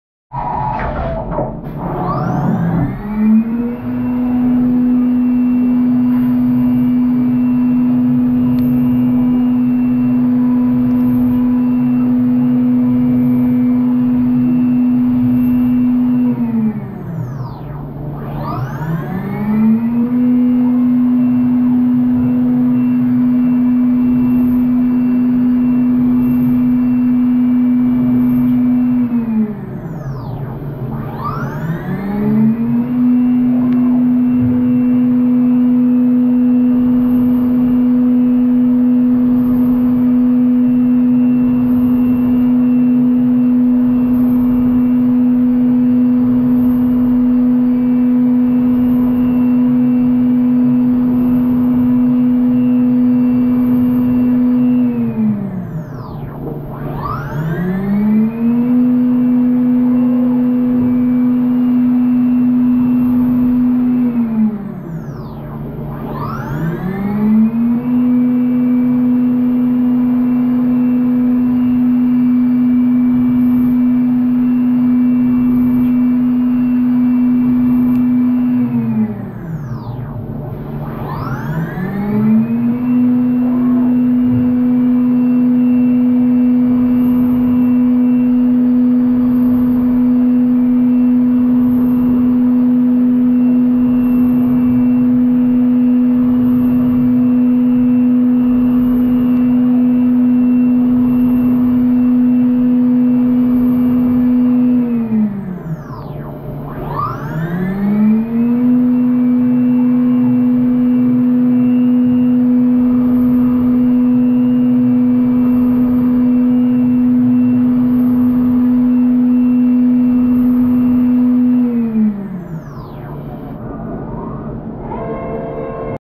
5 axis Drilling on CNC machining center 5 axis OKUMA MU400 VII
Best performance of CoroDrill 860-NM for non-ferrous materials, such as aluminium alloys, magnesium and copper based alloys including bronze.